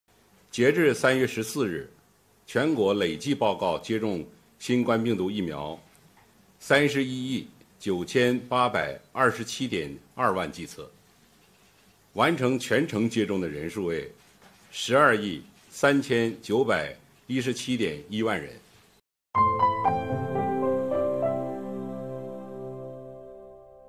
国家卫健委：超12.3亿人完成新冠疫苗全程接种
3月15日，国务院联防联控机制召开新闻发布会。会上，国家卫生健康委新闻发言人米锋通报，截至3月14日，全国累计报告接种新冠病毒疫苗31亿9827.2万剂次，完成全程接种的人数为12亿3917.1万人。